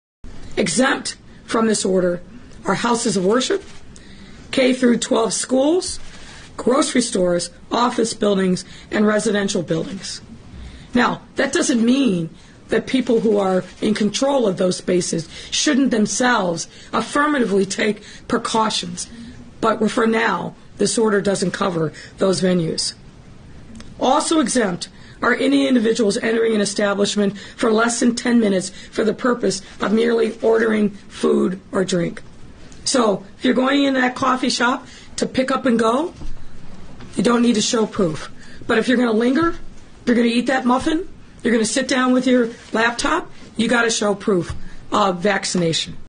Lori Lightfoot explains her insane Covid policies:
Lightfoot: "If you're going into that coffee shop to pick-up and go, you don't need to show proof. But if you're gonna linger, you're gonna eat that muffin, you're gonna sit down with your laptop, you gotta show proof of vaccination."